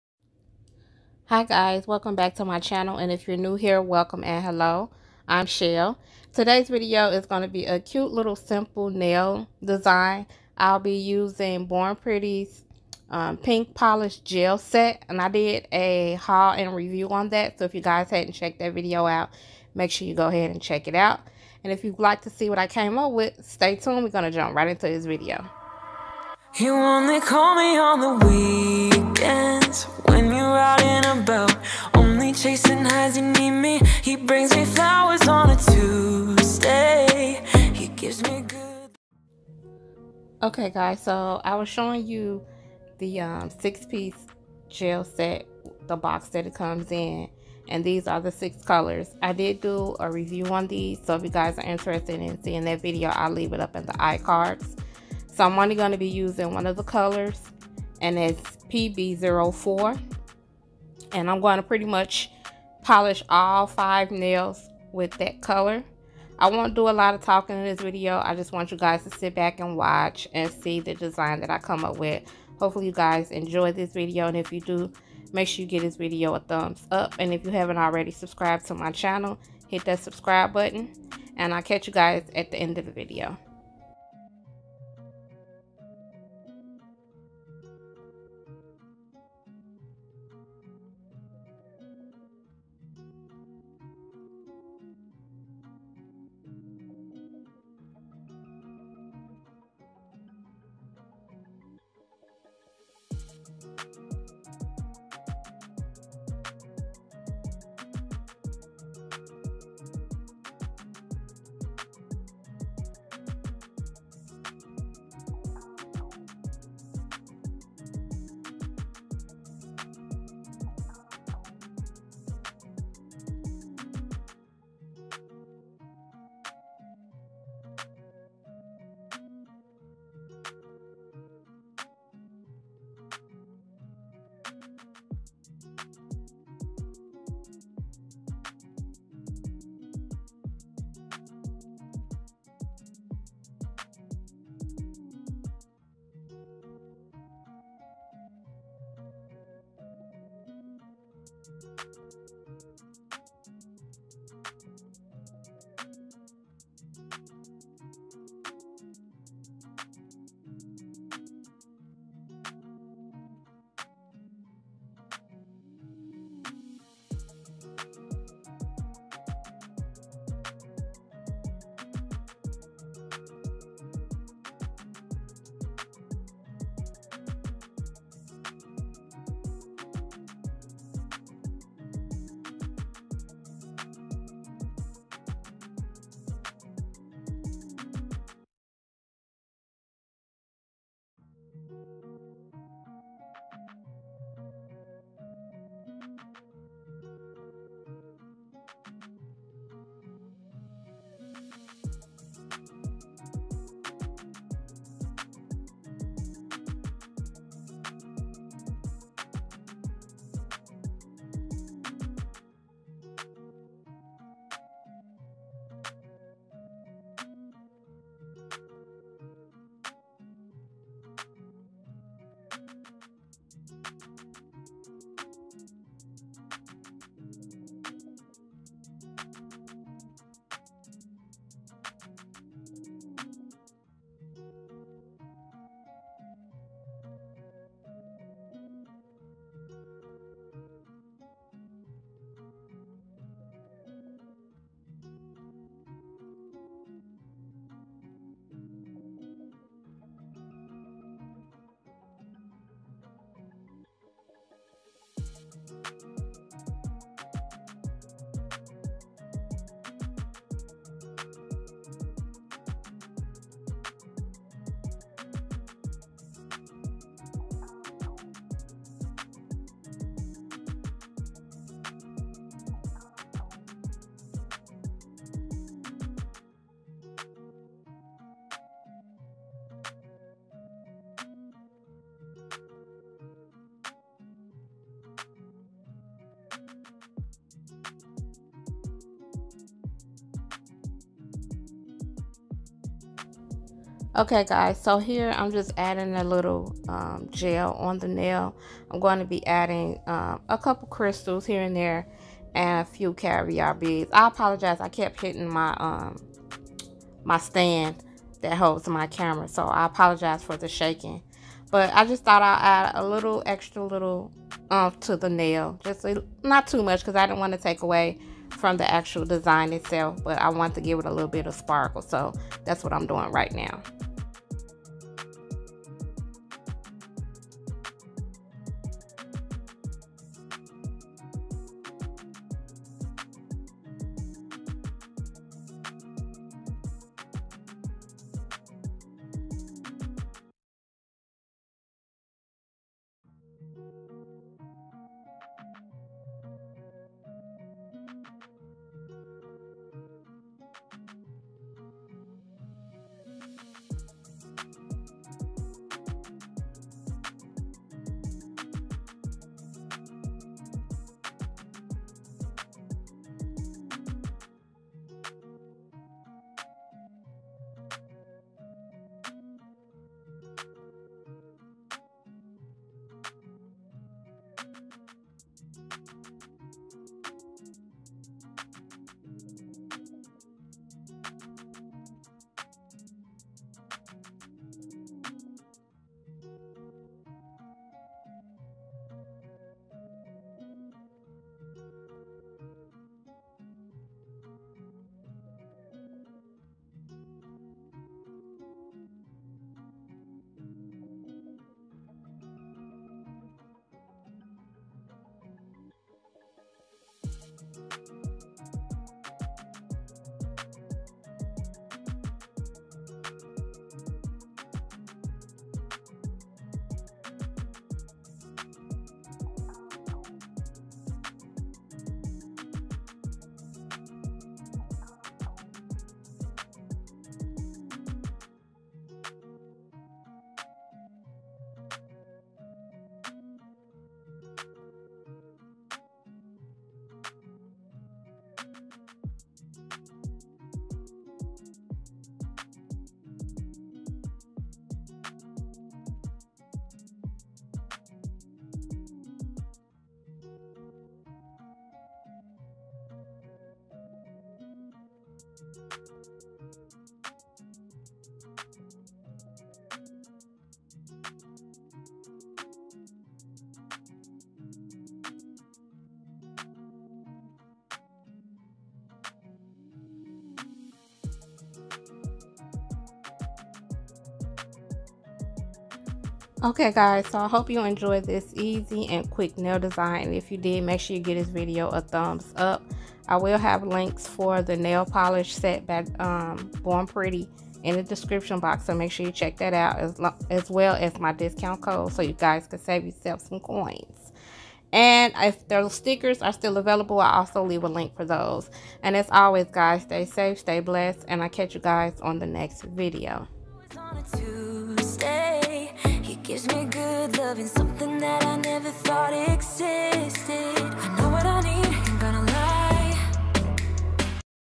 Born Pretty Gel Polish | Simple Nail Art Design | Easy Nail Design
Today, I will be doing a short and easy nail art design, beginner friendly featuring Born Pretty Pink gel polish kit. I hope you enjoy it.

Born Pretty Pink Gel Nail Polish

SHELLZC10

CLARK50

📦Send me gifts:📦
Shellz Couture Nails